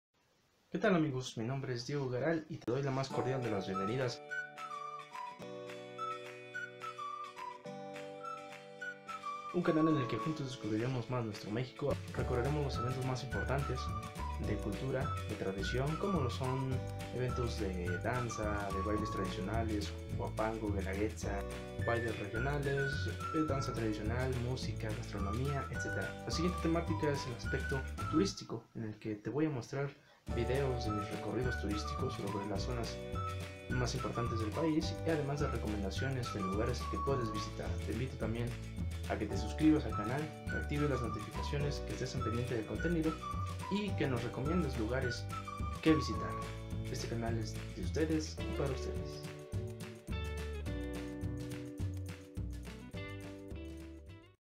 Presentación Del Canal - Diego Go
En este video te cuento un poco del contenido del canal y la manera en que puedes participar haciendo valer tu opinión.

NOTA: Si me ayudas subiendo el volumen, te lo agradecería mucho.